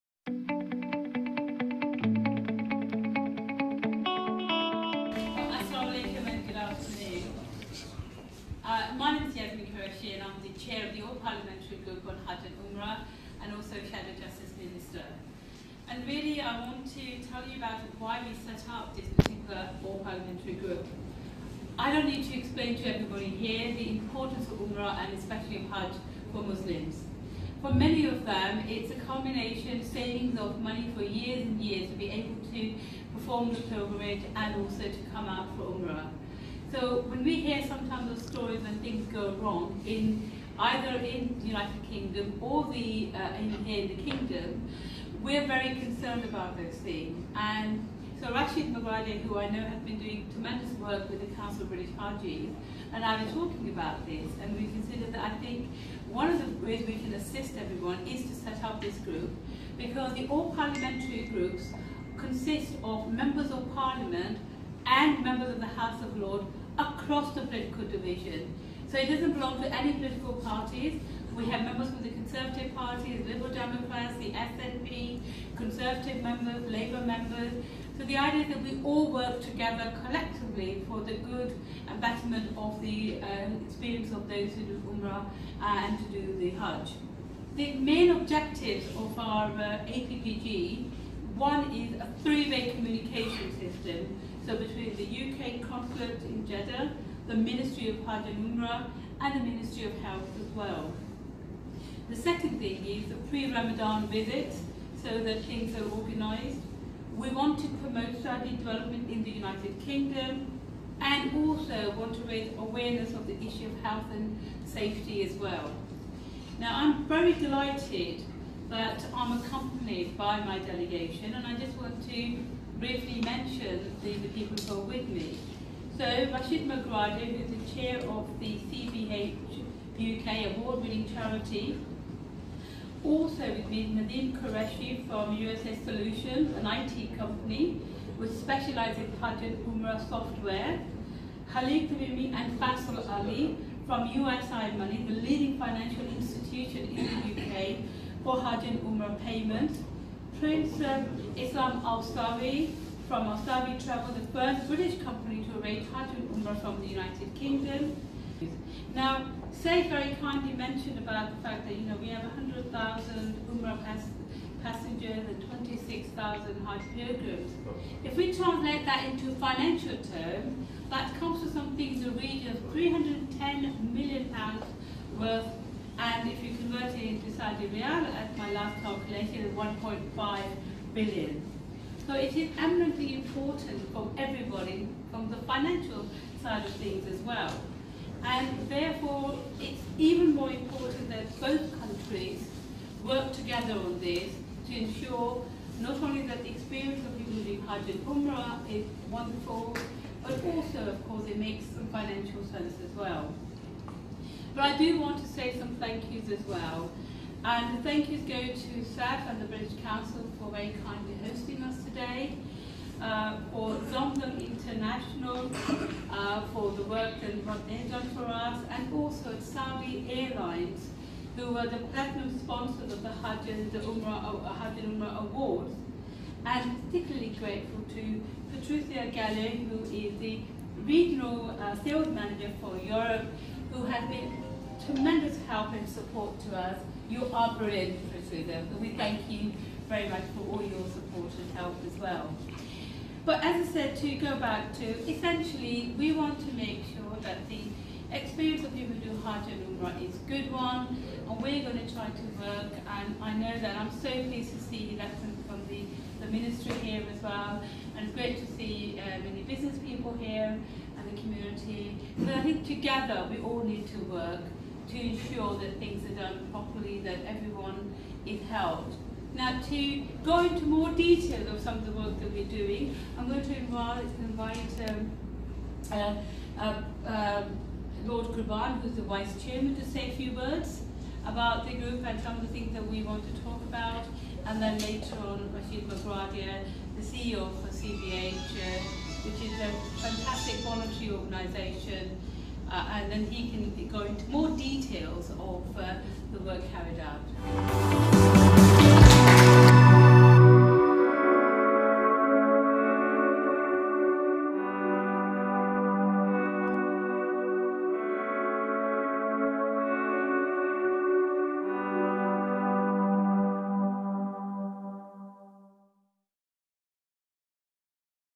Yasmin Qureshi MP's speech at the British Consulate Jeddah KSA 2nd Dec 2018
Yasmin Qureshi MP (Chair, All-Party Parliamentary Group on Hajj & Umrah) speech at the British Consulate Jeddah on 2nd December 2018.

The APPG were  hosted by the British Consul-General.

Coverage in the Arab News:
British delegation praises help for Hajj pilgrims